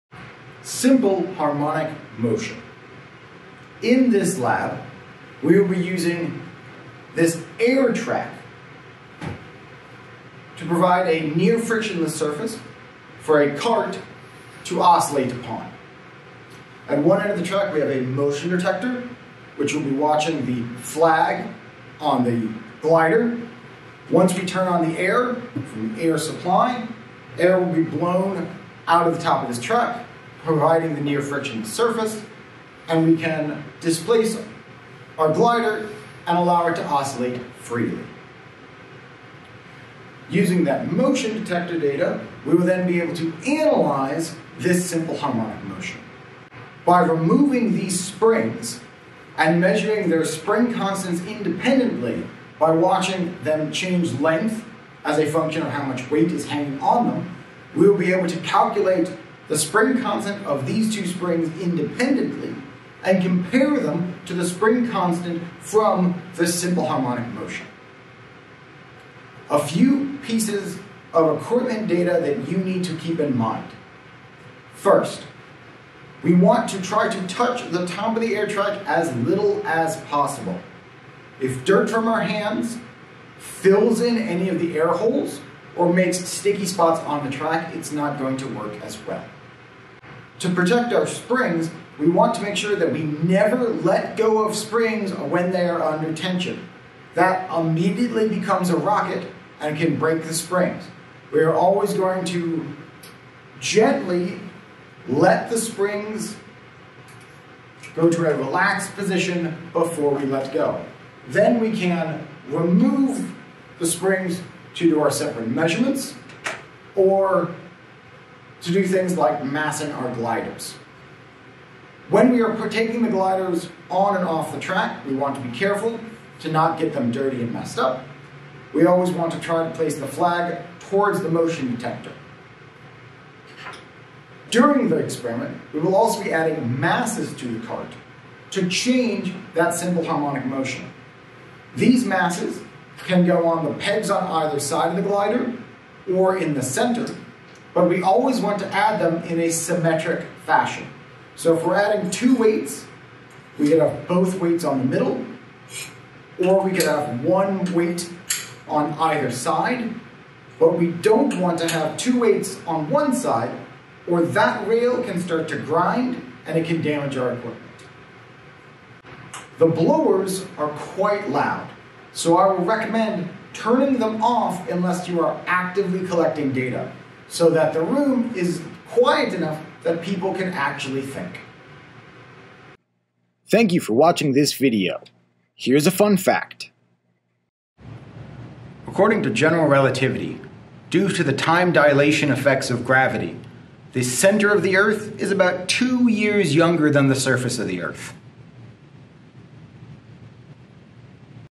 Simple Harmonic Motion
This video is an introduction to the Simple Harmonic Motion lab for PHYS 0101/0150.

If you missed any of the fun facts from this semester, here is the entire collection.